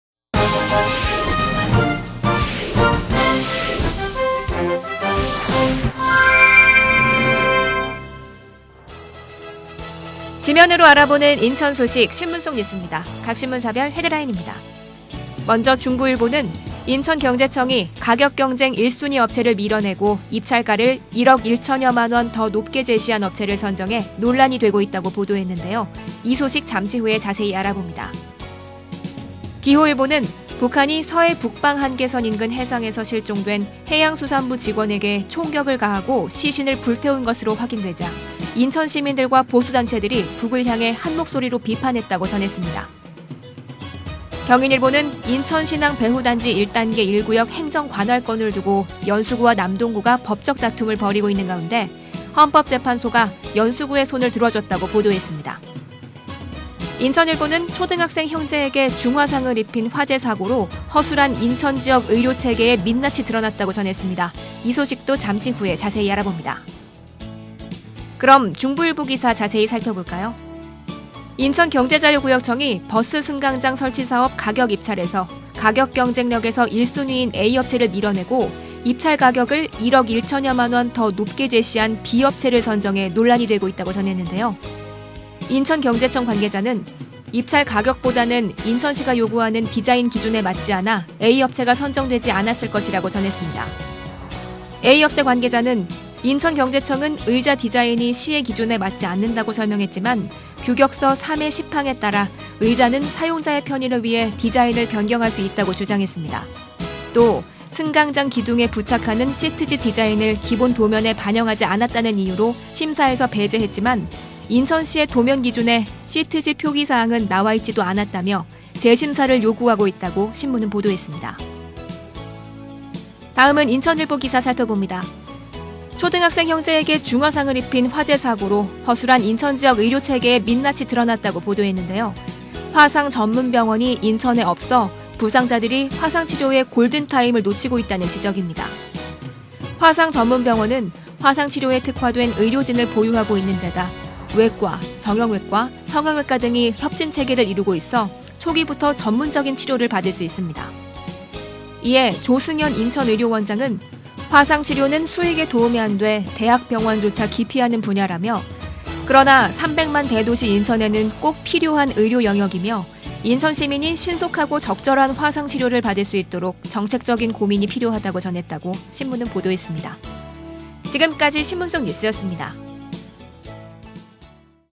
[NIB 뉴스] 신문속뉴스-중부일보, 인천일보
먼저 중부일보는 인천경제청이 가격경쟁 1순위 업체를 밀어내고 입찰가를 1억1천여만 원 더 높게 제시한 업체를 선정해 논란이 되고 있다고 보도했는데요. 이 소식 잠시 후에 자세히 알아봅니다.

기호일보는 북한이 서해 북방한계선 인근 해상에서 실종된 해양수산부 직원에게 총격을 가하고 시신을 불태운 것으로 확인되자, 인천 시민들과 보수단체들이 북을 향해 한목소리로 비판했다고 전했습니다.

경인일보는 인천 신항 배후단지 1단계 1구역 행정 관할권을 놓고 연수구와 남동구가 법적 다툼을 벌이고 있는 가운데 헌법재판소가 연수구의 손을 들어줬다고 보도했습니다.

인천일보는 초등학생 형제에게 중화상을 입힌 화재사고로 허술한 인천지역 의료 체계의 민낯이 드러났다고 전했습니다. 이 소식도 잠시 후에 자세히 알아봅니다.

그럼 중부일보 기사 자세히 살펴봅니다.
1. 인천경제자유구역청이 버스승강장 설치사업 가격입찰에서 가격경쟁력에서 1순위인 A업체를 밀어내고 입찰가격을 1억1천여만 원 더 높게 제시한 B업체를 선정해 논란이 되고 있다고 전했는데요.
2. 인천경제청 관계자는 입찰가격보다는 인천시가 요구하는 디자인 기준에 맞지 않아 A업체가 선정되지 않았을 것이라고 전했는데요.
3. A업체 관계자는 인천경체청은 의자 디자인이 시의 기준에 맞지 않는다고 설명했지만 ‘규격서 3-10항’에 따라 의자는 사용자의 편의를 위해 디자인을 변경할 수 있다고 주장했습니다. 또, 승강장 기둥에 부착하는 시트지 디자인을 기본도면에 반영하지 않았다는 이유로 심사에서 배제했지만, 인천시의 도면 기준에 시트지 표기 사항은 나와 있지도 않았다며 재심사를 요구하고 있다고 신문은 보도했습니다.

다음은 인천일보 기사 살펴봅니다.
1. 초등학생 형제에게 중화상을 입한 화재사고로 허술한 인천지역 의료 체계의 민낯이 드러났다고 보도했는데요. 화상 전문병원이 인천에 없어 부상자들이 화상 치료의 골든타임을 놓치고 있다는 지적입니다.
2. 화상 전문병원은 화상 치료에 특화된 의료진을 보유하고 있는 데다, 외과・정형외과・성형외과 등이 협진체계를 이루고 있어 초기부터 전문적인 치료를 받을 수 있습니다.
3. 이에 조승연 의천의료원장은 “화상치료는 수익에 도움이 안 돼 대학병원조차 기피하는 분야”라며 “그러나 300만 대도시 인천에는 꼭 필요한 의료영역이며, 인천시민이 신속하고 적절한 화상치료를 받을 수 있도록 정책적인 고민이 필요하다”고 전했다고 신문은 보도했습니다.